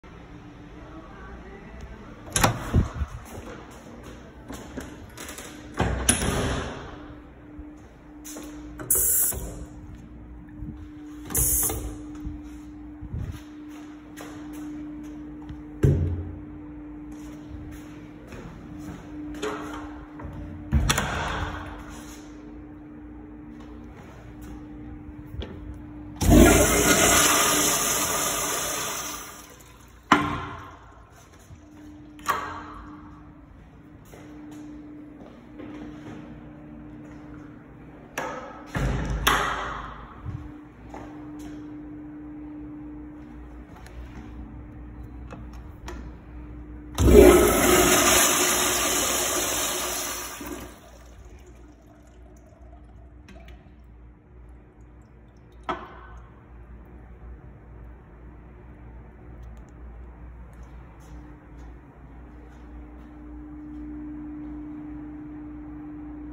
Sausalito #5 West Marine Men's Restroom Full Shoot
West Marine in Sausalito CA off of HWY 101. I only like the toilets.

"Urinal": ❌
Kingstons: A

Rating Scale

A+ EPIC
A Amazing
A- Nice
B+ Great
B Good
B- Okay
C+ Boring
C Not Good
C- Bad
D+ Junk
D Garbage
D- Trash
E+ Terrible
E Horrible
E- Awful